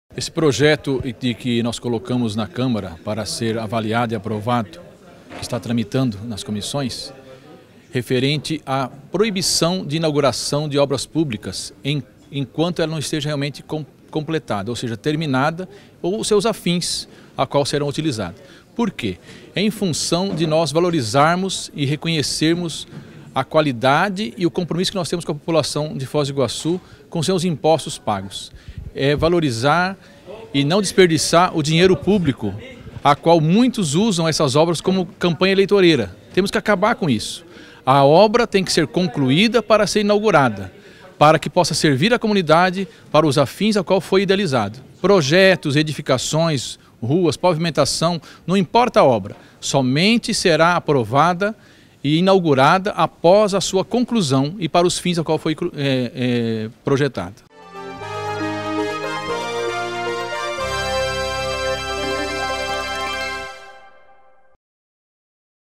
Projeto proíbe a inauguração de obras públicas enquanto não estiverem finalizadas
O Vereador Celino Fertrin apresentou um projeto, na sessão desta terça-feira (04), que proíbe a inauguração de obras públicas enquanto não estiverem finalizadas.